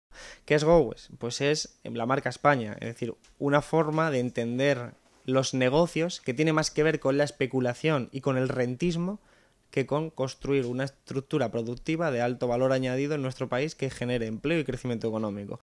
Garzón (IU): "Gowex es un reflejo extremo de cómo funcionan las empresas en este país"
El portavoz económico de Izquierda Unida en el Congreso de los Diputados, Alberto Garzón, manifestó que la empresa Gowex "es un reflejo y un símbolo extremo de cómo están funcionando las empresas en este país".

Así lo indicó el también responsable de la Secretaría Ejecutiva del Proceso Constituyente de IU en una entrevista concedida a Servimedia, al ser preguntado por el escándalo financiero de Gowex.
"Gowex es especulación, trampas, relaciones con el poder político, subvenciones, comidas en despachos y no es inversión. Eso es un reflejo y un símbolo extremo de cómo están funcionando las empresas en este país", manifestó.